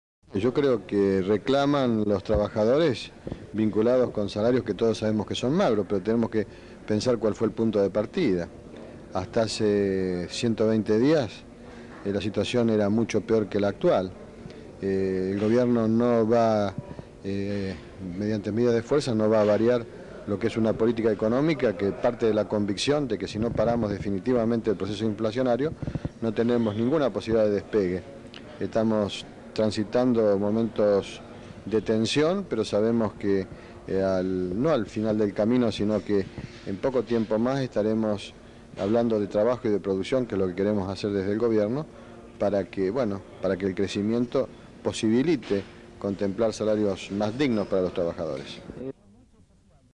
Reportaje a Eduardo Duhalde sobre reclamo salarial (1989)
As.: Reportaje al vicepresidente Eduardo Duhalde donde habla del reclamo salarial de los trabajadores para afrontar el proceso inflacionario.
Fecha: 7/11/1989
Duración: 51 segundos
Código: UG-1500